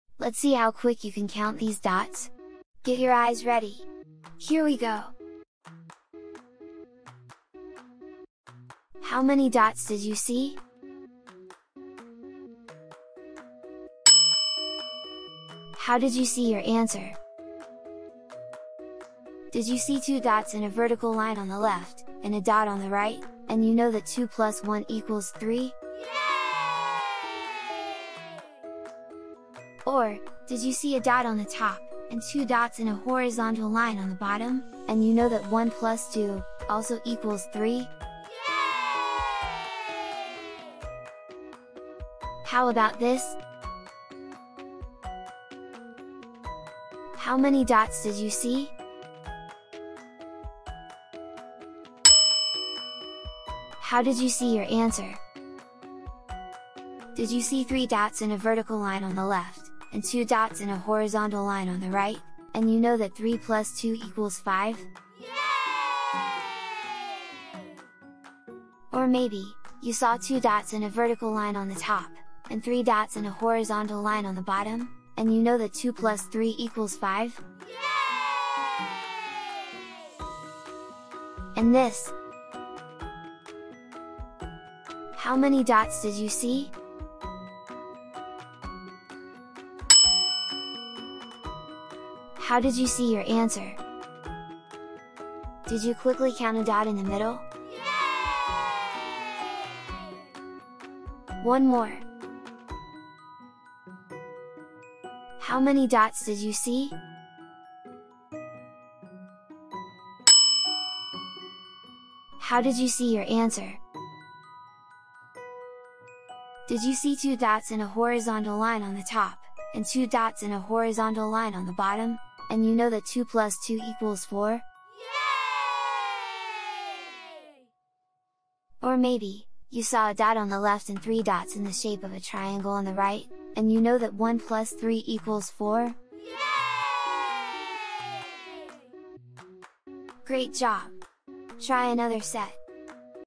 FUN Addition activity- HOW MANY DOTS DO YOU SEE?
Level I Set 2
Pre-K to 5th grade Number Talks!
Start building fluency and becoming a young mathematician!
Mental math using dot images.